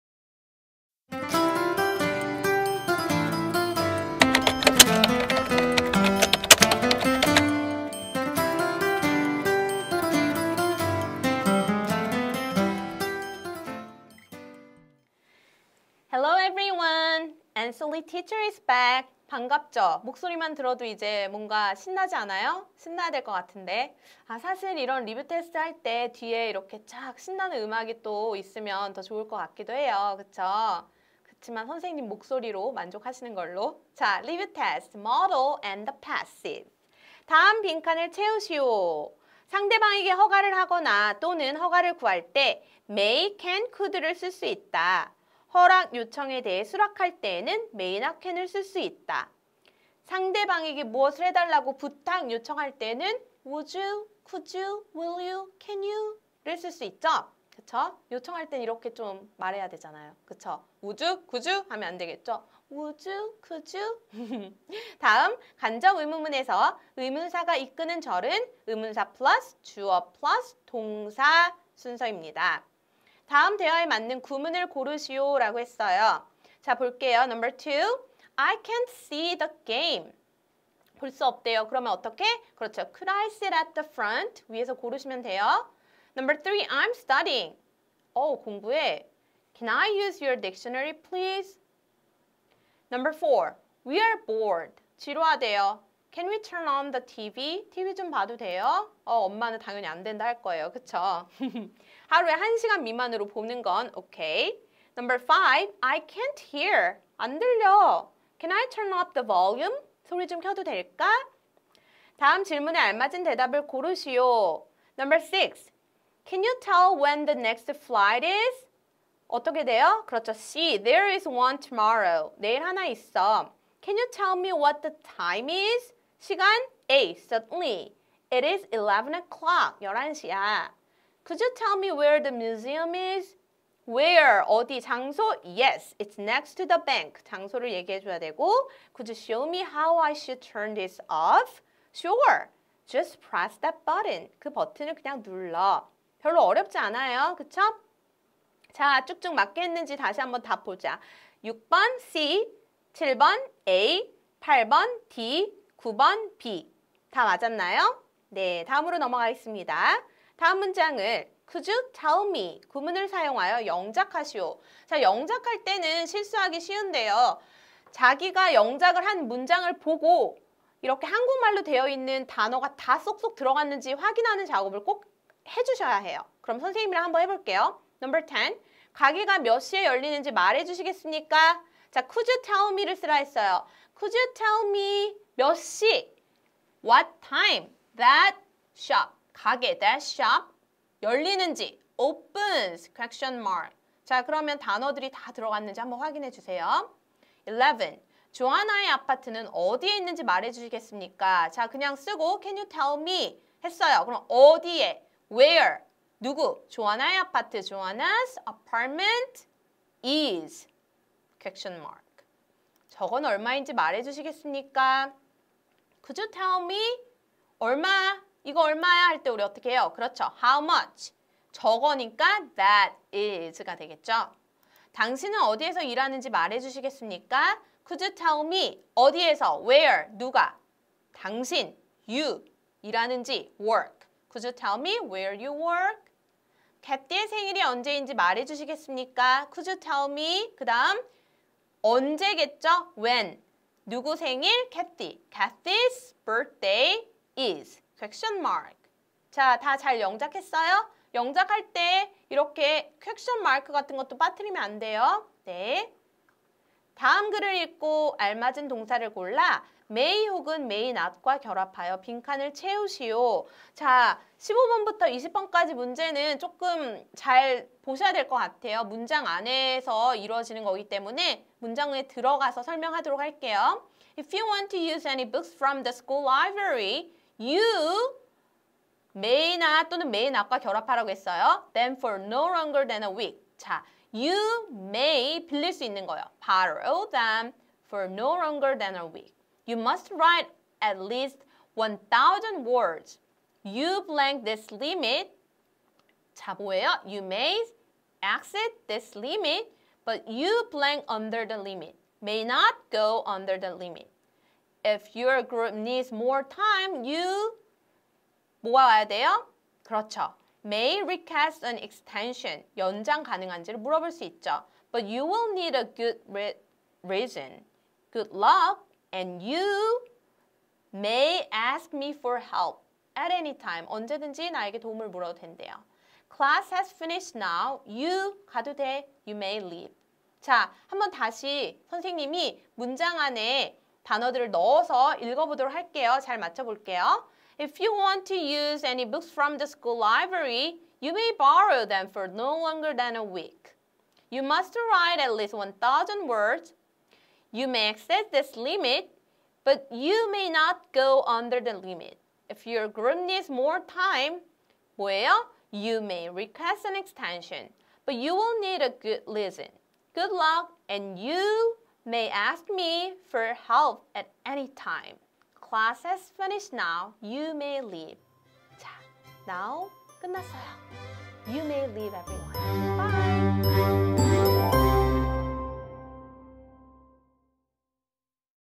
Grammar High 3-11 Modals3
♠️♠️초등 Daily Test 문제풀이♠️♠️
(Grammar High 3_L11_Modals(3))

정답 콕콕~ 해설 술술~~ 설명해주는
Answer Lee 선생님과 함께하는
초등 Daily Test 문제풀이 시간입니다^^

초등 5학년 친구들 같이 풀어볼까요~?